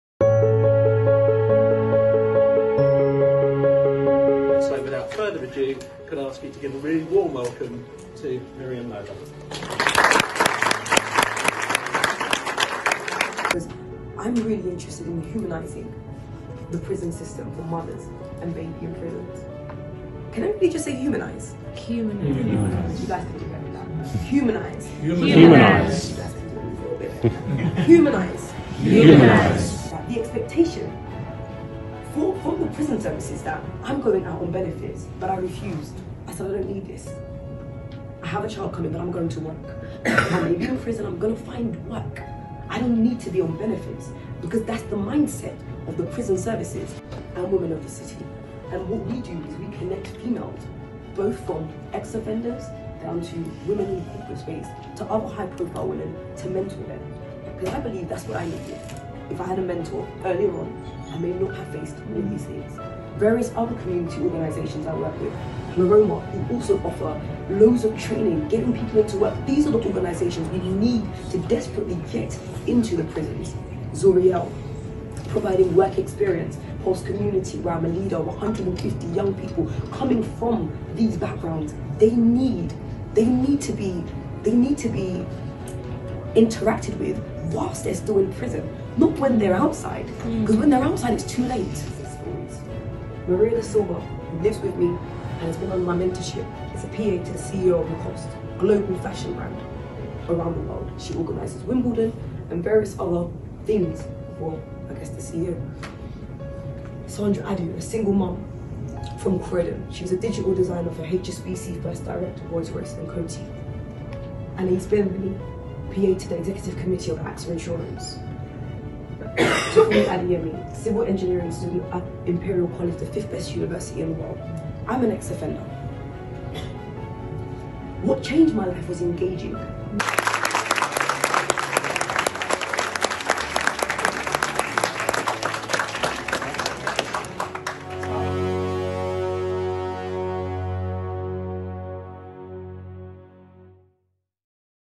Mariam Mola Hosts The National Family Conference for HM Prison & Probation Service
Mariam Mola hosted this year’s National Family Conference for HMP & Probation Services where she spoke alongside the Simon Marshall Deputy Director rehabilitation & support services group, HM Prison & Probation Service, Diane Caddle Executive Director Prison Safety & Rehabilitation HM Prison & Probation Service, Dr Angela Christopher MB, FSET National Families Advisor HM Prison & Probation Service, Andy Keen- Downs CEO of PACT, Polly Wright Chair of The Bristol & South Gloucestershire Steering Group On Children Affected By Parental Offending and Dr Samantha Callan policy expert in family relationships, family law, early years, mental health and domestic abuse and parliamentary advisor to Lord Farmer.

Mariam’s talk was on the needs of women in prison and the benefits of female prisoners receiving continuous family support whilst incarcerated.

Mola, who has come from a background within the prison system, shared her story on how she was able to overcome this through the power of mentorship.

The conference was filled with numerous governors, directors, service providers and partners within the prison field, all with the aim of further discussing and learning about the most efficient routes and solutions in favour of family contact and support.